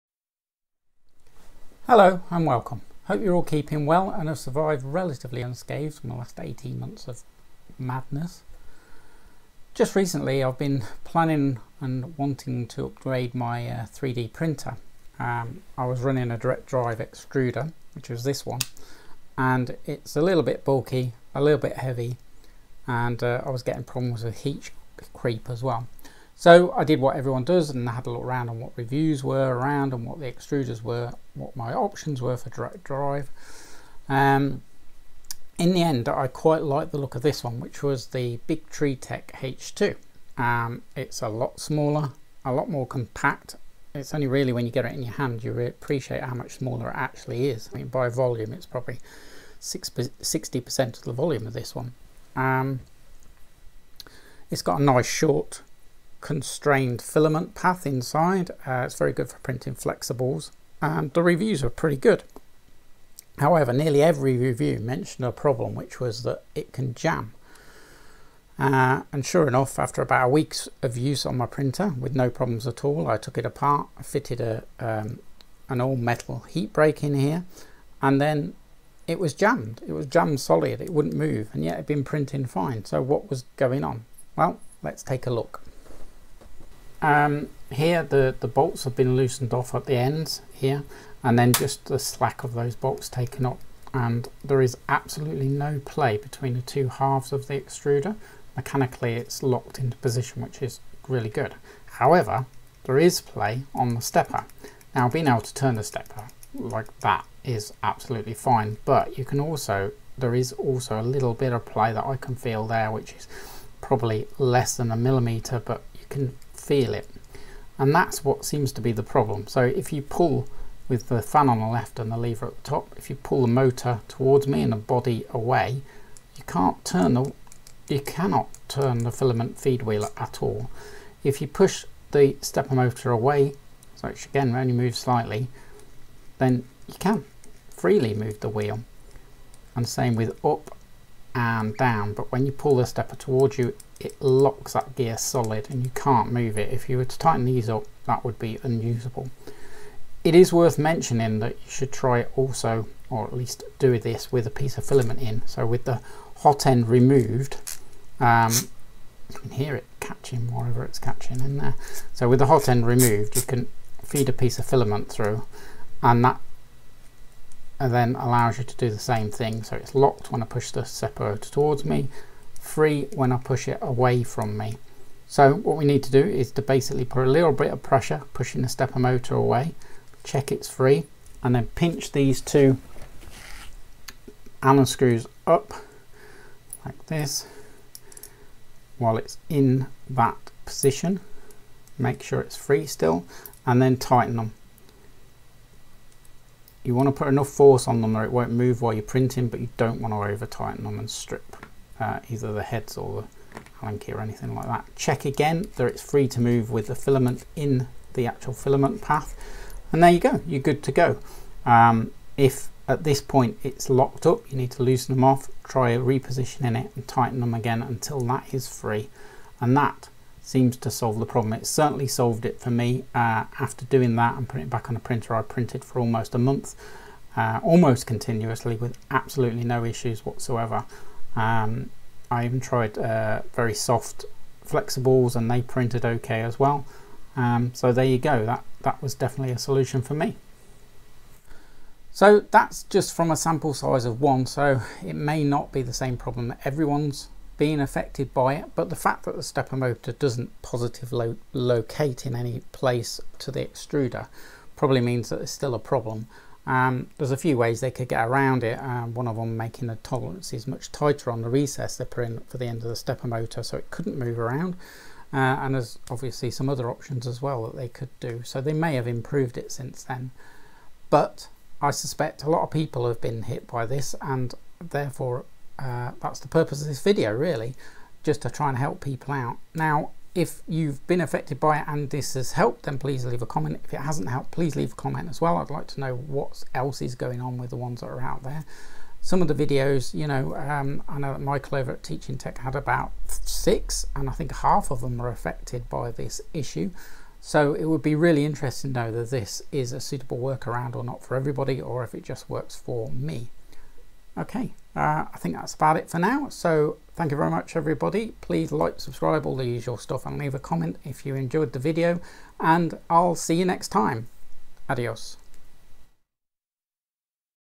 How to fix a jammed biqu H2 extruder
These biqu H2 extruders are great, but they do tend to Jam, this is how I sort that out when it happens to me. This stops it jamming altogether.

ordering
cheapest for uk